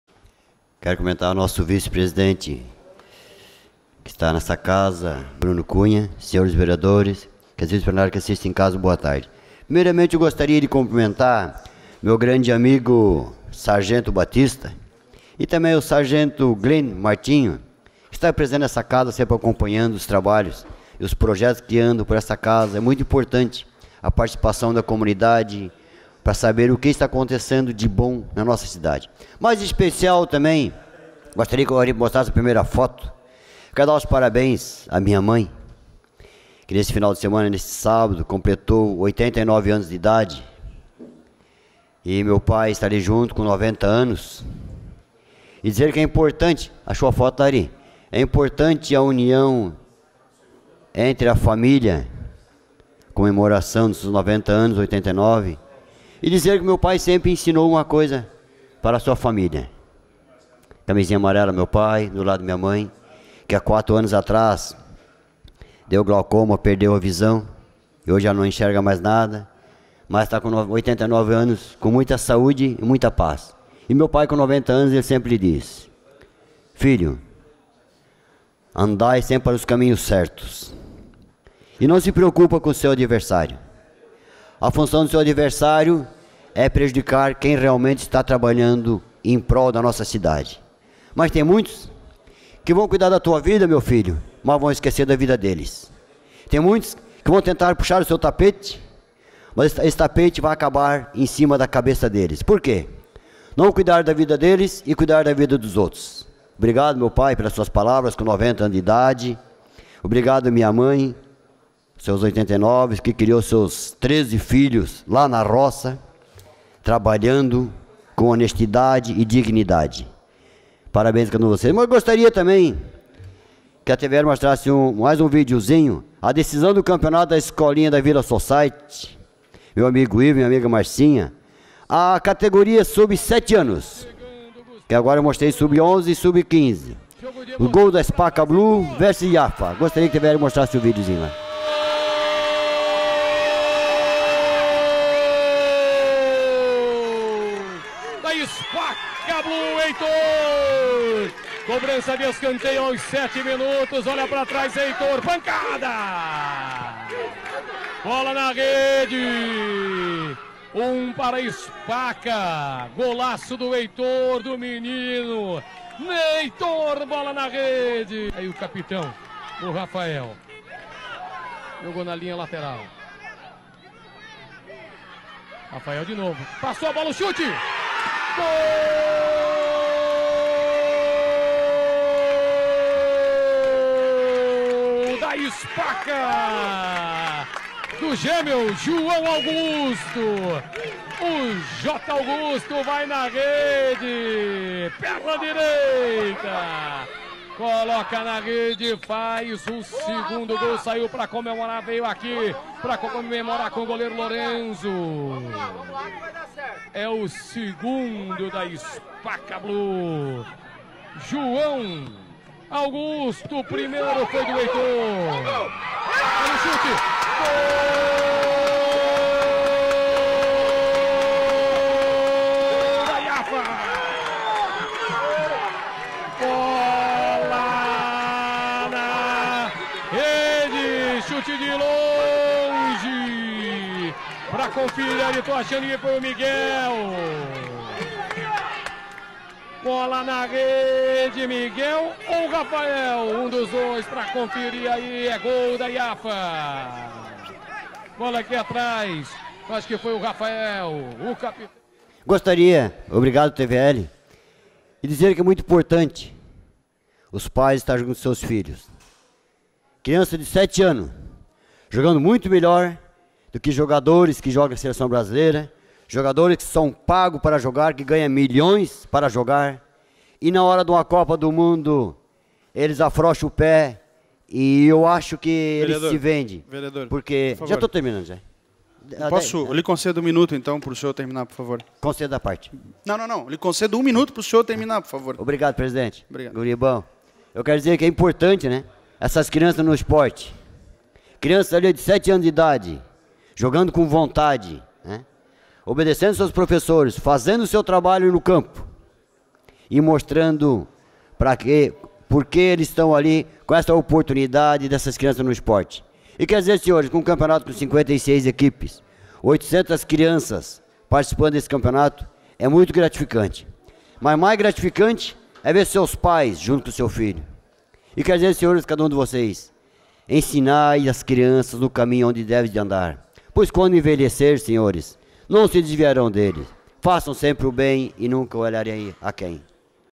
Pronunciamento Vereador Oldemar Becker - 30/07/2019
Vereador Oldemar Becker destaca importância de projetos sociais voltados à formação do caráter das crianças
O vereador Oldemar Becker (DEM) exibiu o vídeo do campeonato de escolinhas de futebol sub-7, realizado no Vila Society. Destacou a vitória da Spaca em cima da IAFA. Falou ainda sobre o envolvimento dos pais na vida das crianças que muitas vezes jogam com mais garra e determinação do que jogadores que ganham milhões de reais. Comentou que é gratificante ver as crianças aprendendo a andar pelos caminhos do bem. Em outro momento, parabenizou sua mãe que completou 89 anos de idade com muita saúde, ao lado de seu pai com 90 anos. Sustentou a importância da união da família. Afirmou que seu pai sempre ensinou a família a andar pelos caminhos certos, sem se preocupar com seus adversários.